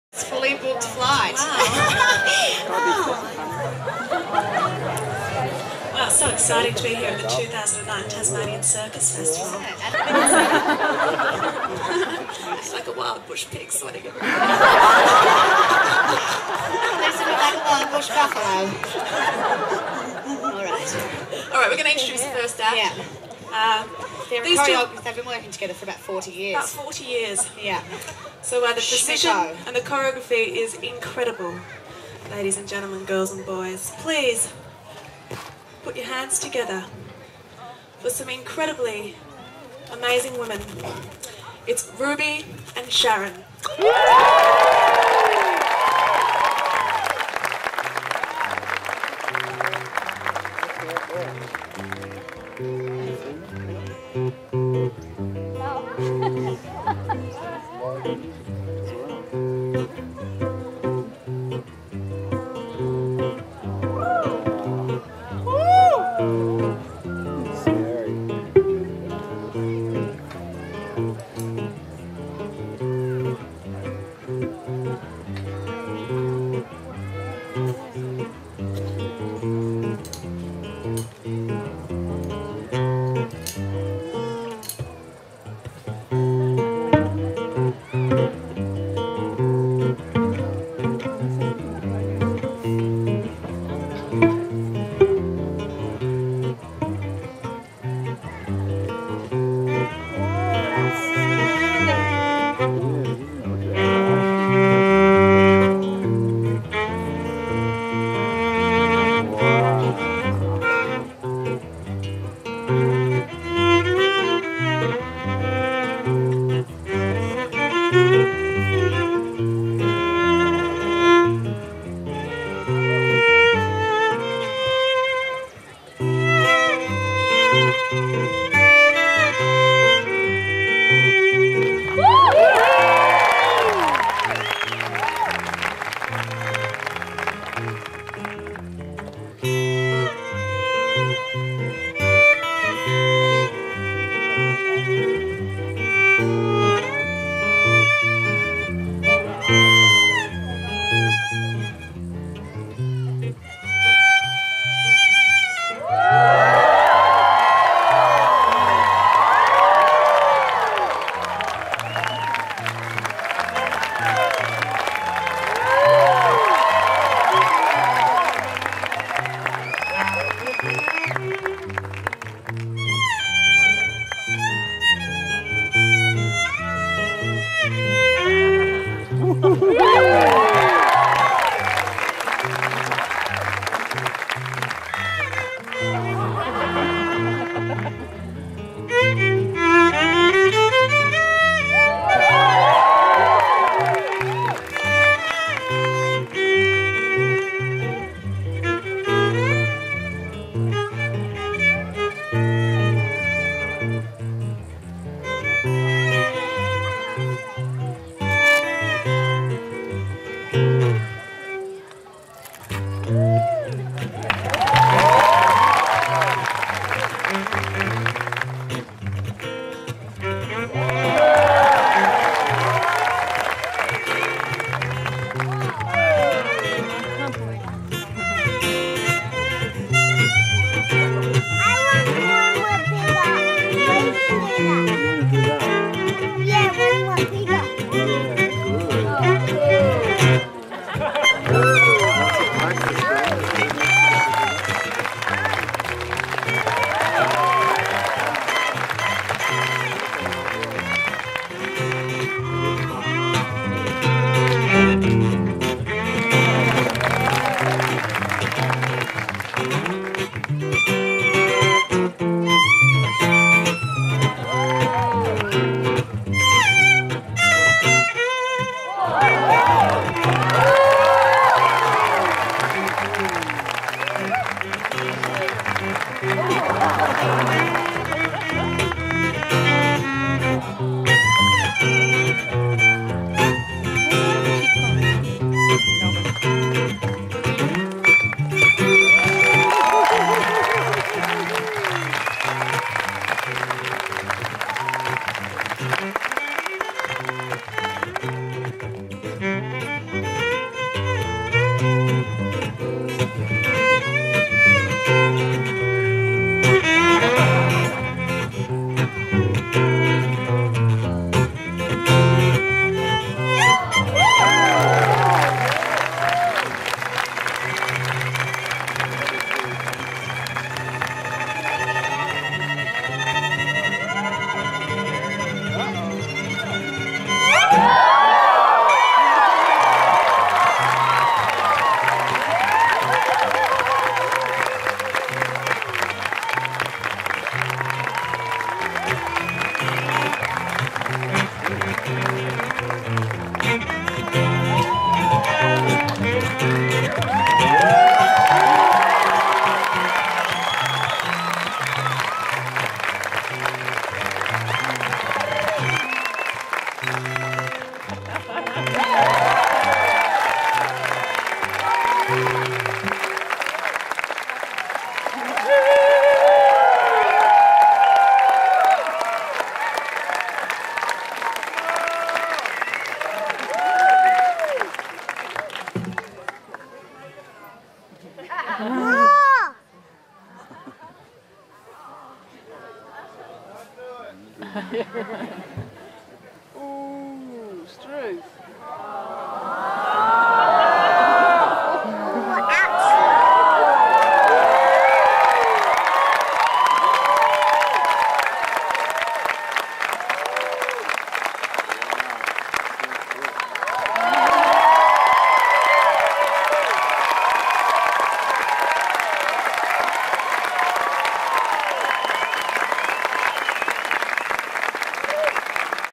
RubySharonTazDuo
2 artists hanging out on the double trapeze, performing at the Tasmania Circus Festival. Act made with one hour on the gear, just for fun!  impeccable lines, no?